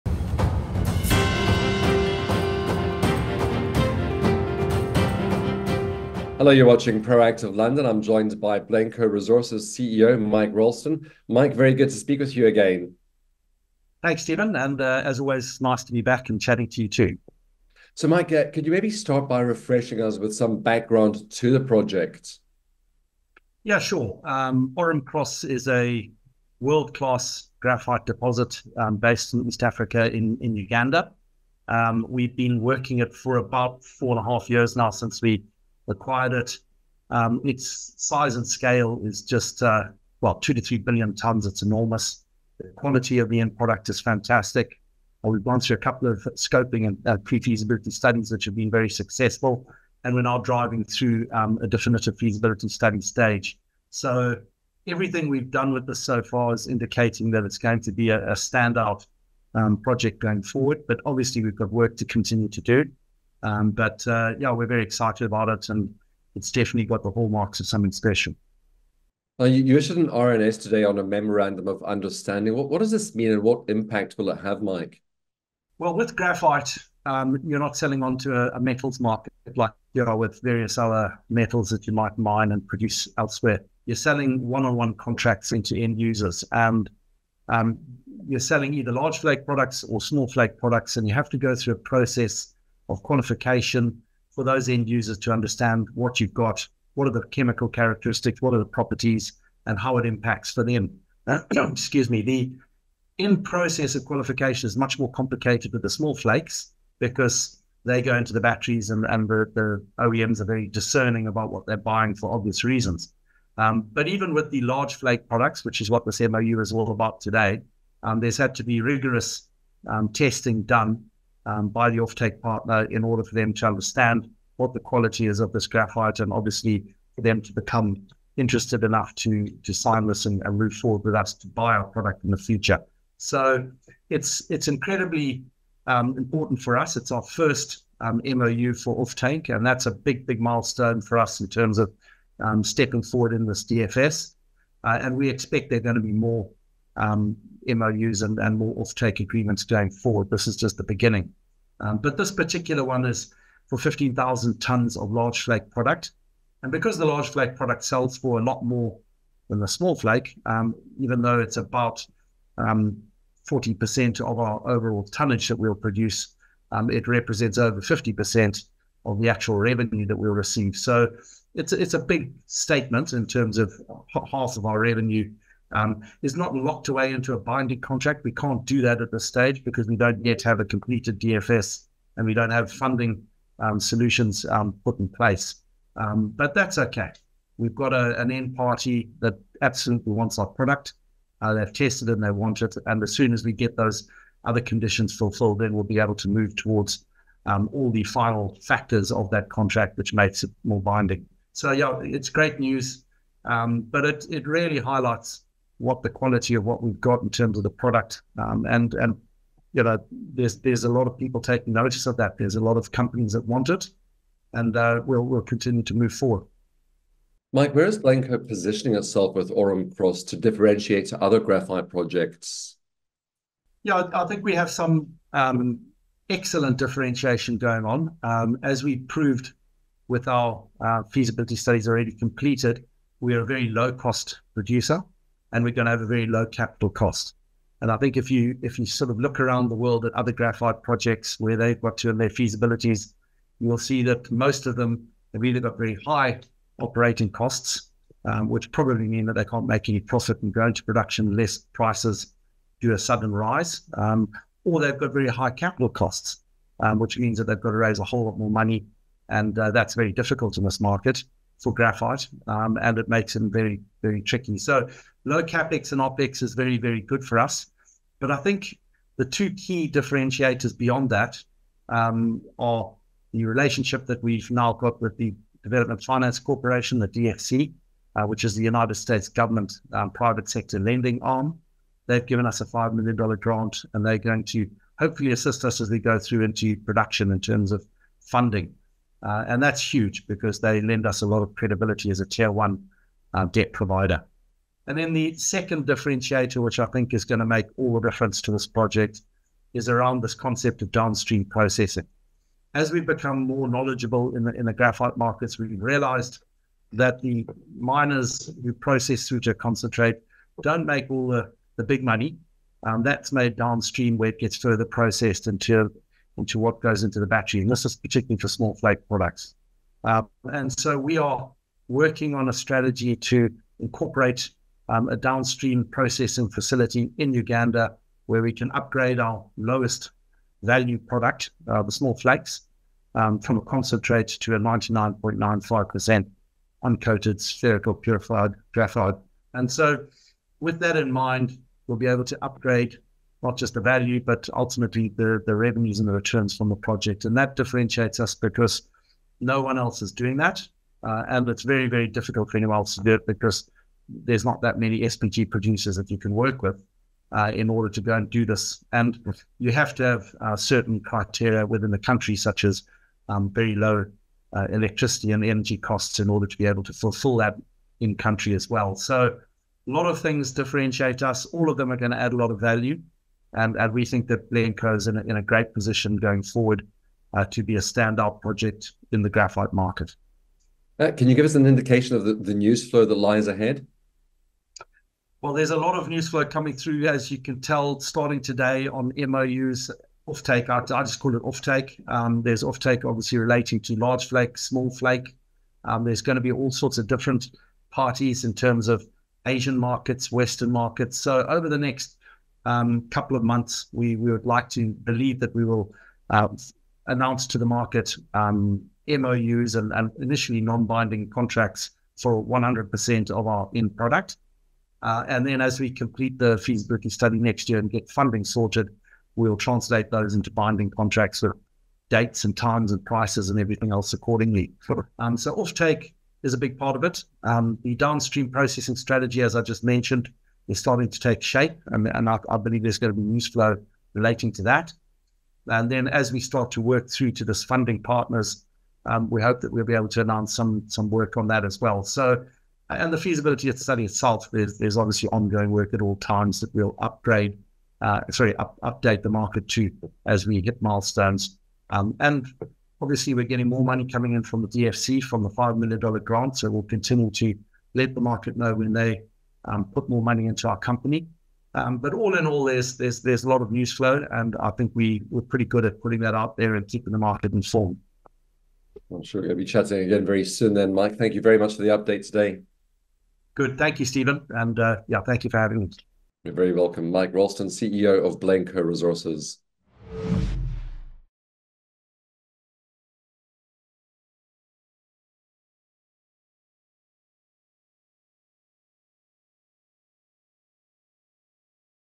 Blencowe Resources hits big milestone with first offtake deal at Orom-Cross
Blencowe Resources PLC (LSE:BRES) CEO Mike Ralston tells Proactive's Stephen Gunnion the company has reached a significant milestone with the signing of its first memorandum of understanding (MOU) for natural large flake concentrate offtake from the Orom-Cross graphite project in Uganda.

Ralston provided insights into the project’s scale, its high-quality graphite deposit in Uganda, and the progress made over the past four and a half years.

He explained the rigorous testing processes for its large flake and small flake graphite products, emphasising the importance of the large flake product, which represents over 50% of their projected revenue despite being only 14% of their total tonnage.

The CEO also outlined Blencowe’s strategic differentiators, including low operational and capital costs, a $5 million grant from the US Development Finance Corporation (DFC), and plans for downstream processing to add value. Ralston expressed optimism about future news flow, including additional MoUs, the feasibility study completion, and securing funding.